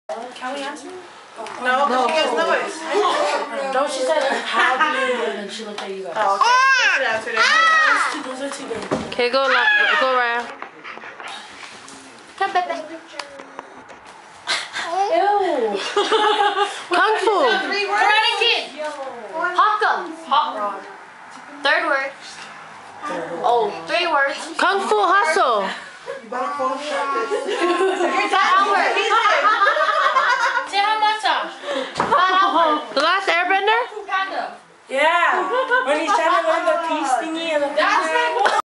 Sariah Suapaia playing Charades!
my little cousin Sariah taking her turn in Charades!